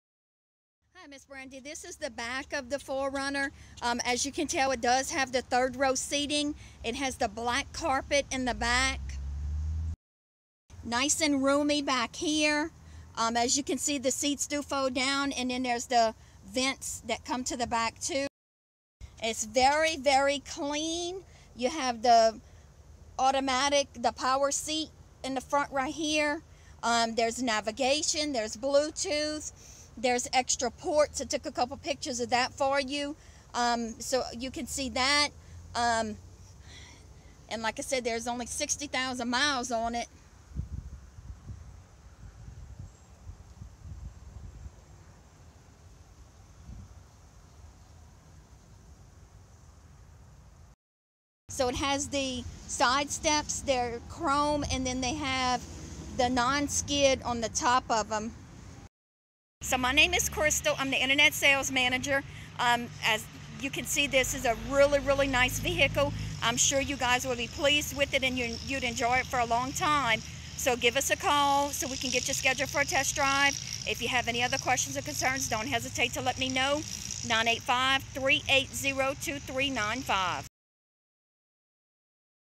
Pre-owned 2013 Toyota 4Runner Limited Stock
This 2013 4Runner is in EXCELLENT CONDITION! It's truly a MUST SEE!  It has just over 60,000 miles and is loaded with features--Bluetooth, NAV, extra ports for phones, USB & Auxillory ports, 12V plug in the console, 3rd row seating, 2nd row bench seat with split & folds down, air vents for back seat, running boards and much more!  Give us a call or come on by 1105 Brashear Ave in Morgan City to take it out for a spin.